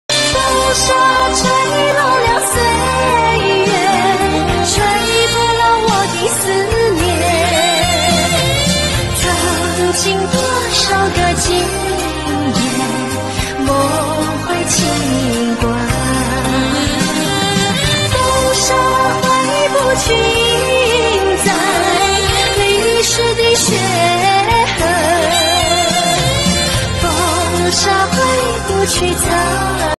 EPARK mini bumper car，car model bumper car，multifunctional bumper car
Model NO.:EP-B003
Size:L145*W95*H67 CM
Product Weight: 80KG
Max Loading Weight: 100KG
Motor: 48V 500W
1.8A/1P battery charger
48V 500W Battery, can use 100-200 mins
Color: Red, Orange, Blue, Green
With seat belt
Player: 1
Battery:48V 500W
Work time:100-200 mins